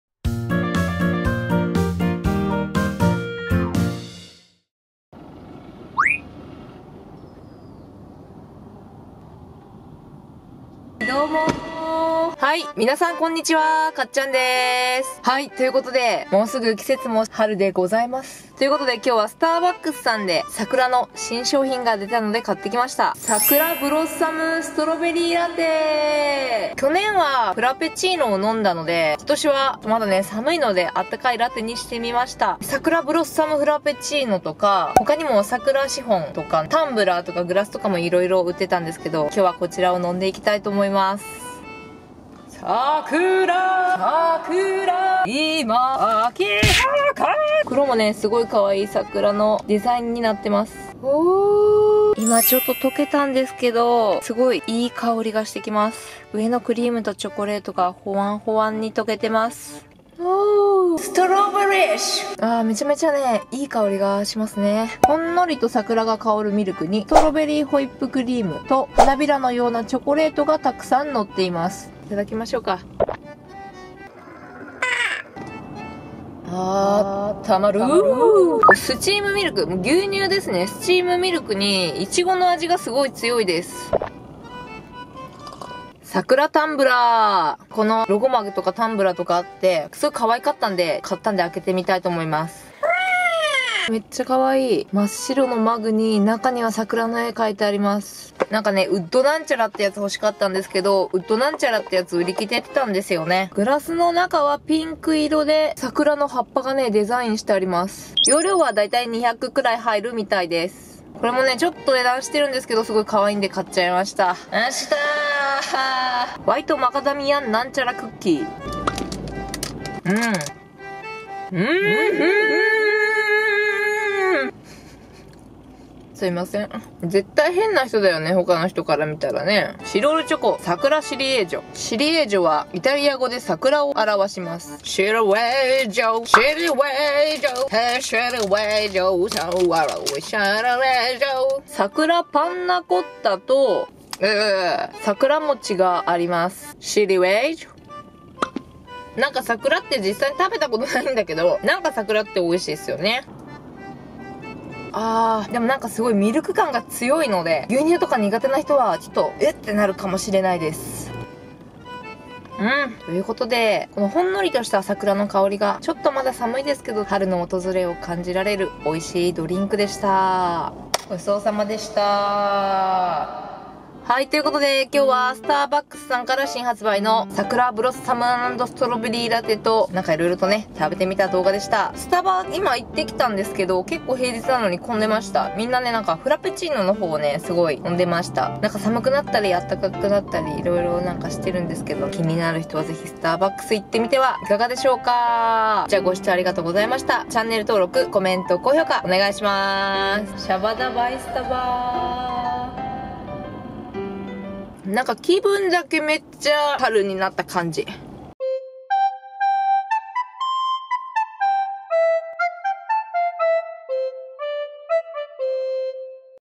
【スタバの春】「さくらブロッサム＆ストロベリーラテ」と「さくらロゴマグ2016」
スターバックスコーヒーさんから春先取りの新メニュー「さくら ブロッサム＆ストロベリー フラペチーノ」と「さくら ブロッサム＆ストロベリー ラテ」が2月15日(月)から始まっていたので早速飲みに行って来ました！